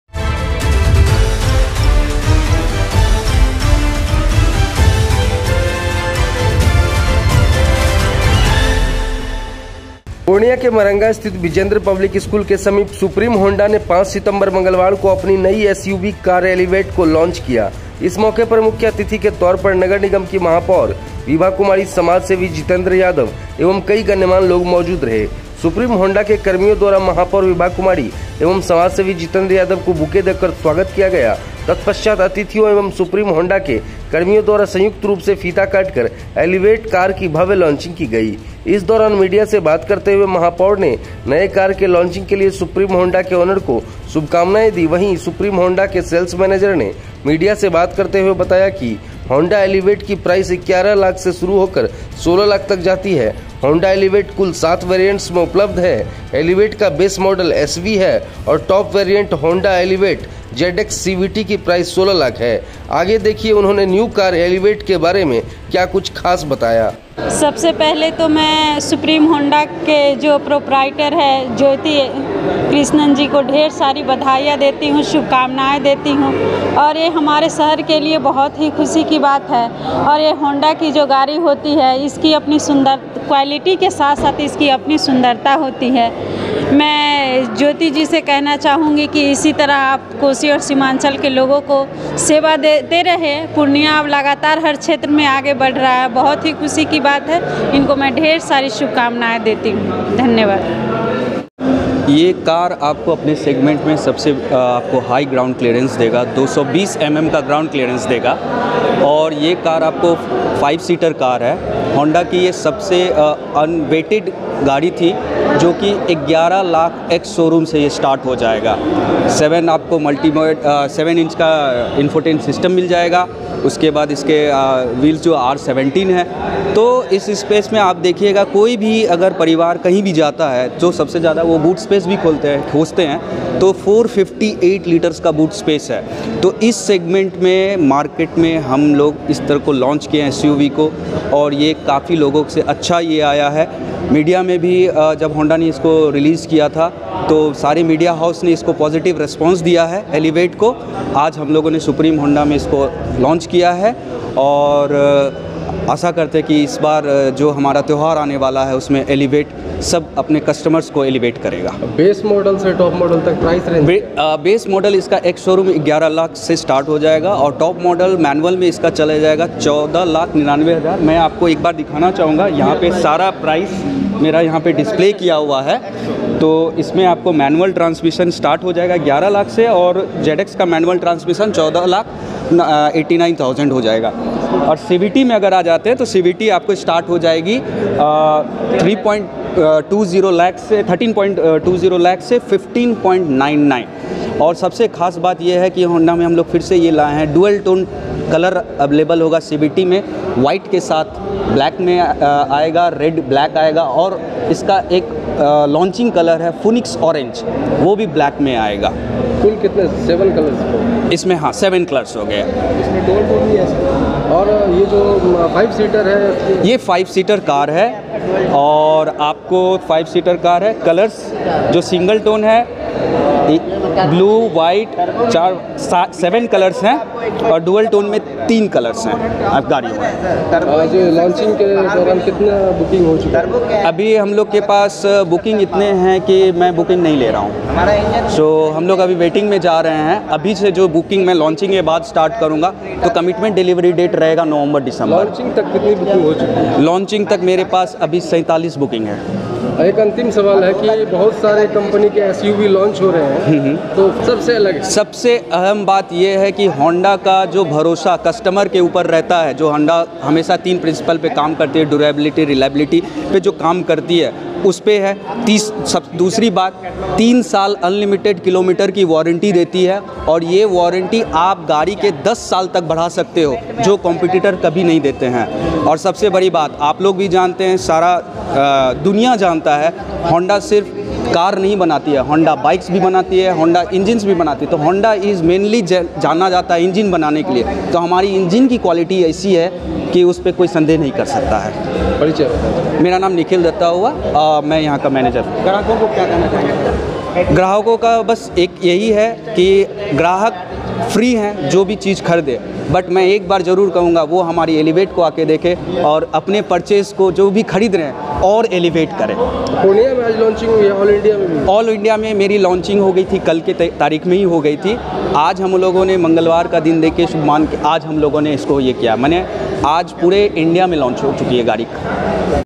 होंडा की नई एसयूवी कार एलीवेट के लांचिंग में ग्राहकों की उमड़ी भीड़।
पूर्णिया के मरंगा स्थित बिजेंद्र पब्लिक स्कूल के समीप सुप्रीम होंडा ने 5 सितंबर मंगलवार को अपनी नई एसयूवी कार एलीवेट को लांच किया।इस मौके पर मुख्य अतिथि के तौर पर नगर निगम की महापौर विभा कुमारी समाजसेवी जितेंद्र यादव एवं कई गण्यमान्य लोग मौजूद रहे।सुप्रीम होंडा के कर्मियों द्वारा महापौर विभा कुमारी एवं समाजसेवी जितेंद्र यादव को बुके देकर स्वागत किया गया।तत्पश्चात अतिथियों एवं सुप्रीम होंडा के कर्मियों द्वारा संयुक्त रूप से सीता काटकर एलीवेट सव कर की भव्य लांचिंग की गई इस दौरान मीडिया से बात करते हुए महापौर ने नए कार के लांचिंग के लिए सुप्रीम होंडा के ओनर को शुभकामनाएं दी।वहीं सुप्रीम होंडा के सेल्स मैनेजर ने मीडिया से बात करते हुए बताया कि होंडा एलिवेट की प्राइस 11 लाख से शुरू होकर 16 लाख तक जाती है।होंडा एलिवेट कुल 7 वेरिएंट्स में उपलब्ध है। एलिवेट का बेस मॉडल एसवी है और टॉप वेरिएंट होंडा एलिवेट जेडएक्स सीवीटी की प्राइस 16 लाख है।आगे देखिए उन्होंने न्यू कार एलिवेट के फीचर के बारे में क्या कुछ खास बताया।